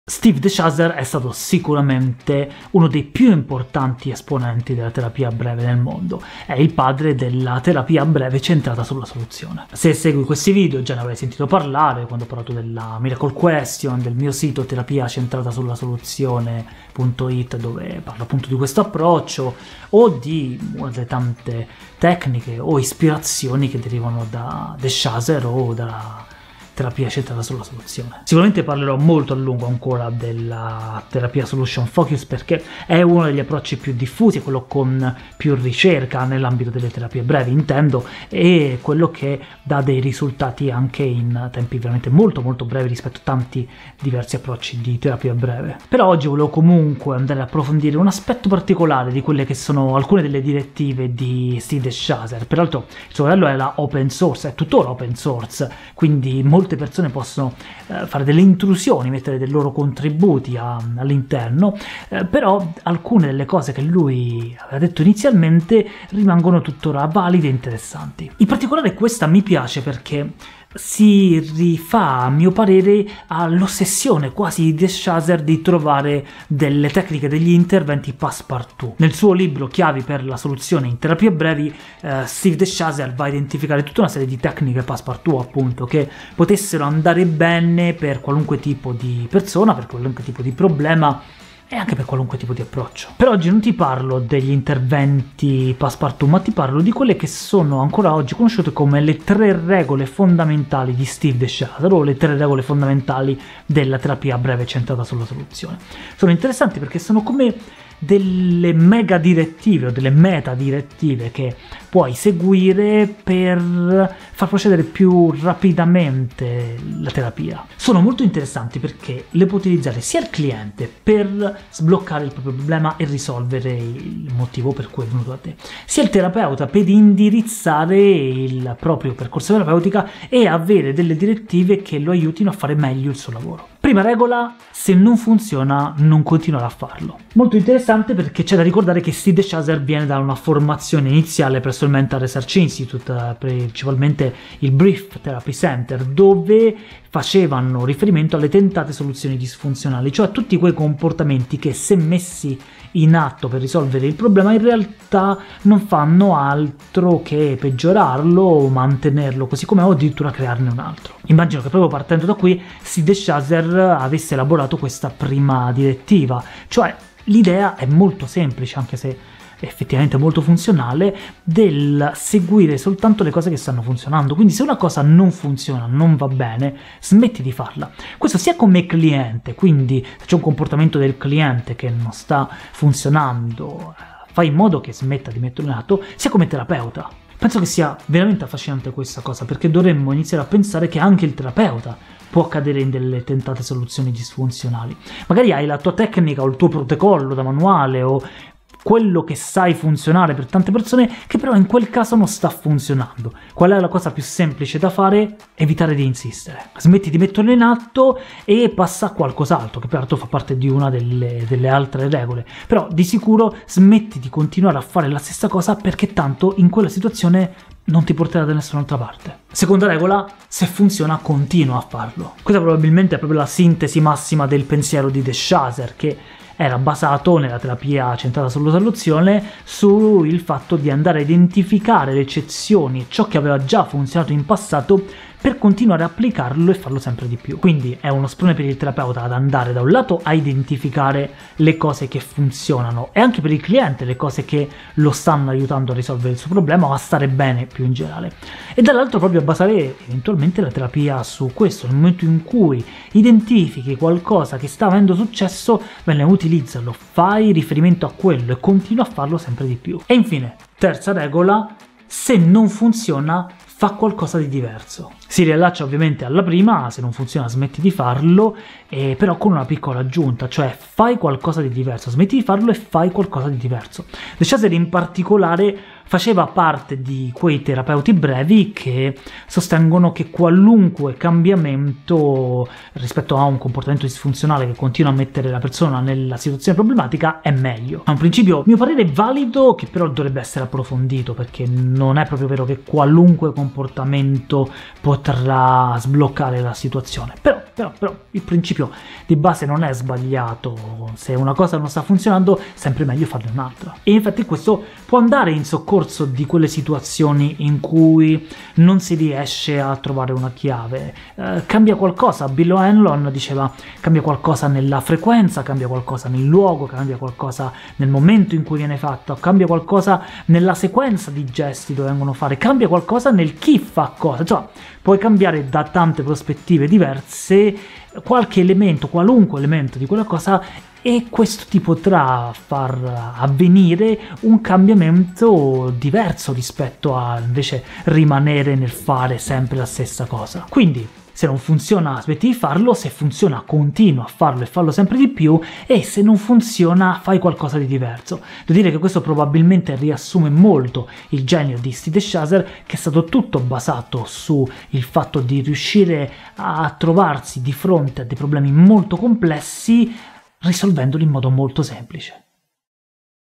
3 regole fondamentali della Terapia Centrata sulla Soluzione
3 Semplici regole da tenere sempre a mente per migliorare l'efficacia della propria terapia!